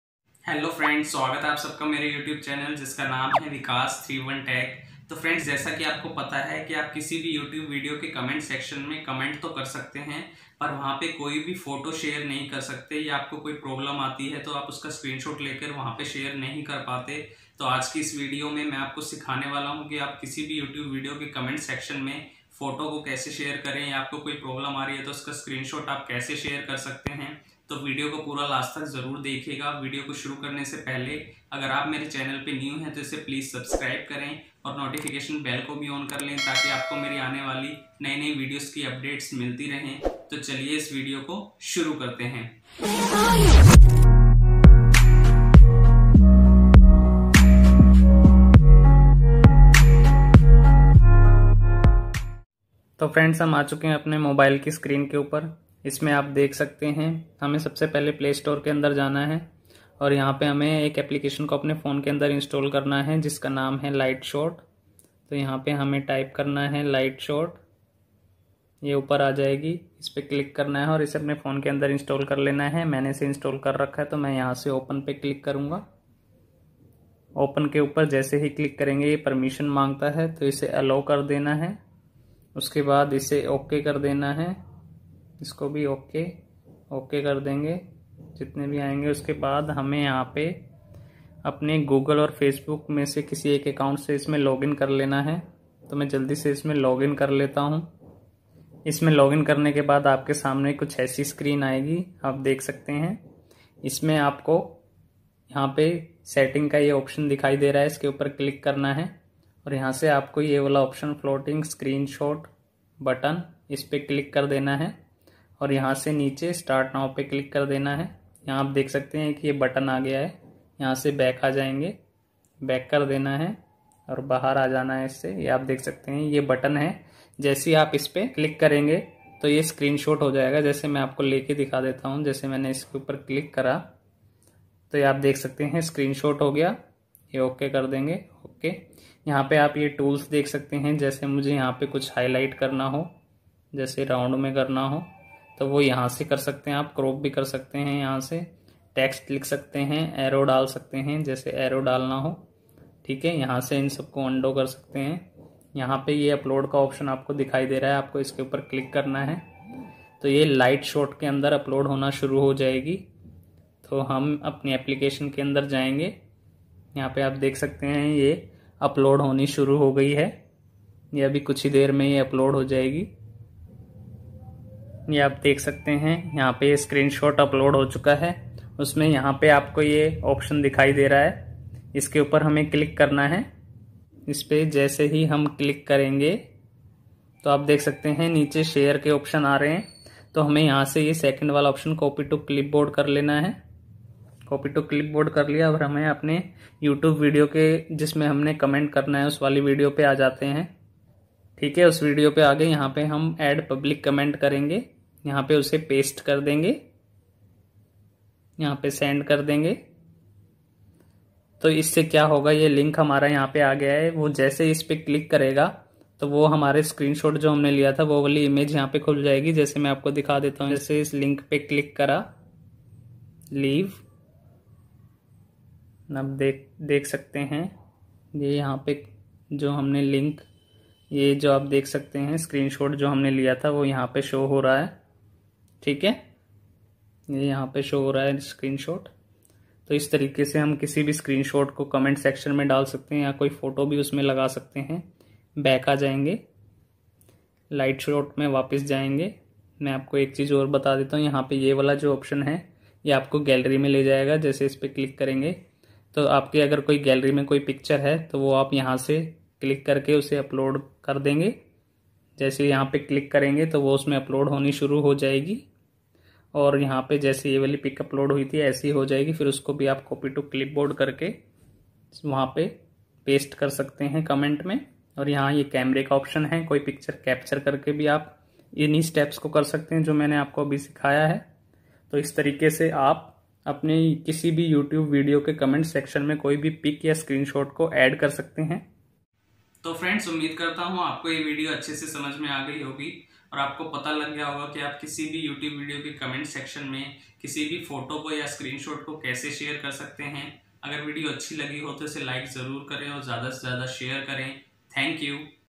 Youtube comment me photo kaise lagaye 2021 | How to comment pictures on youtube 2021
Dosto, is video me maine apko sikhaya hai ki aap youtube ke comment box me kisi bhi photo ya screenshot ko kaise share kar sakte hai.

दोस्तों, इस वीडियो में मैंने आपको सिखाया है कि आप यूट्यूब के कमेंट बॉक्स में किसी भी फोटो या स्क्रीनशॉट को कैसे शेयर कर सकते है |